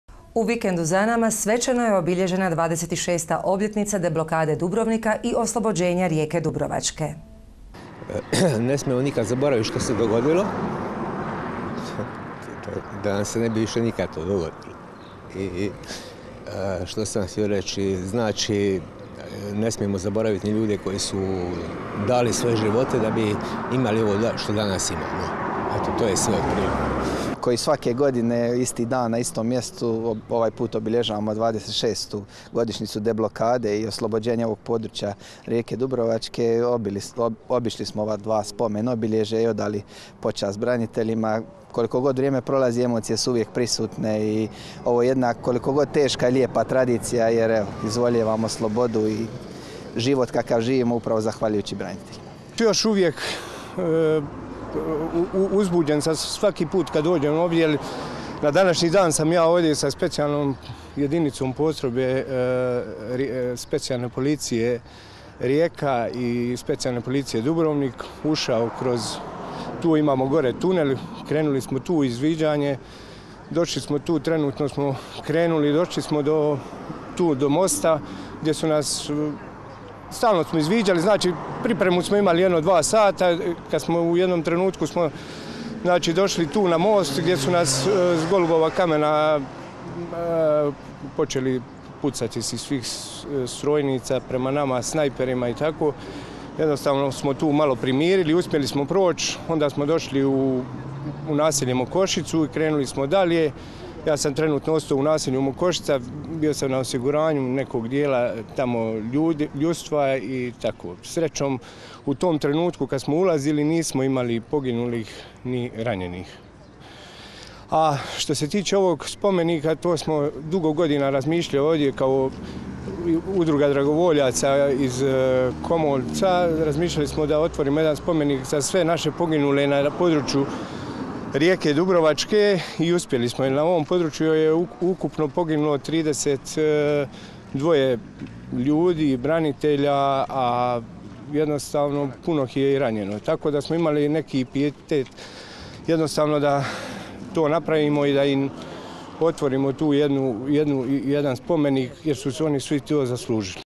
26. obljetnica deblokade Dubrovnika i Rijeke dubrovačke
U vikendu za nama svečano je obilježena 26. obljetnica deblokade Dubrovnika i Rijeke dubrovačke